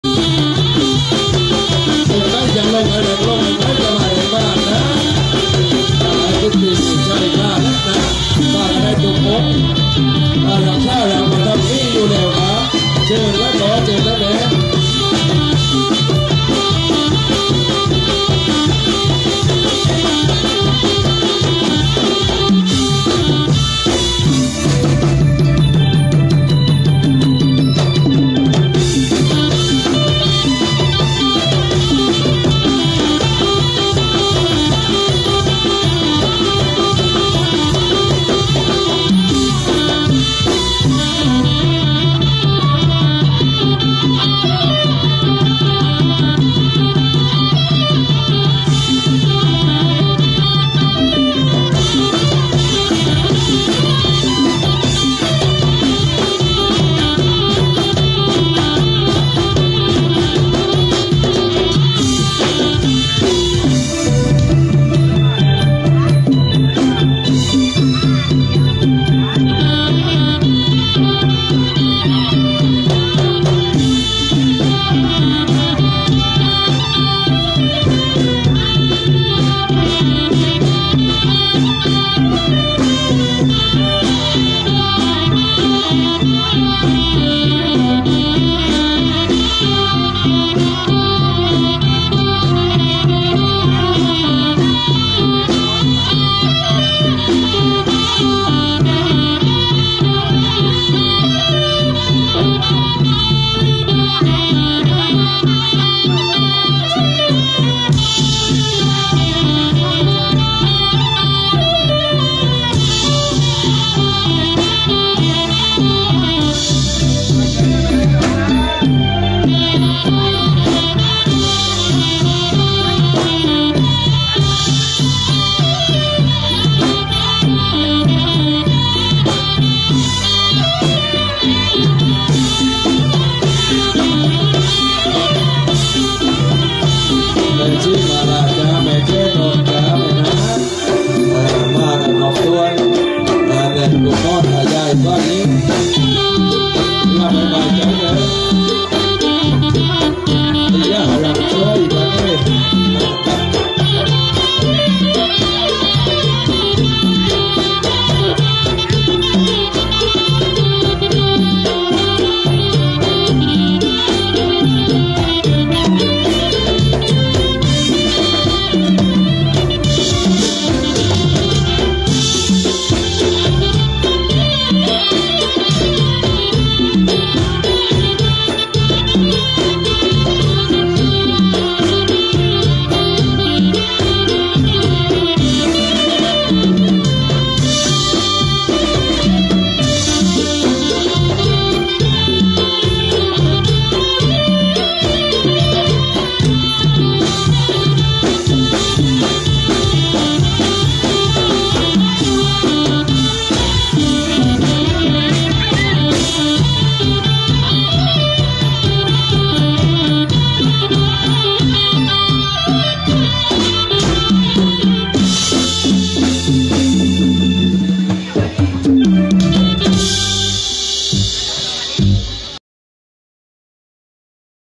ครูหมอประทับทรง รำให้ลูกหลานดู เพื่อเป็นสิริมงคล.
คณะมโนราห์ภิญโญ สองหิ้ง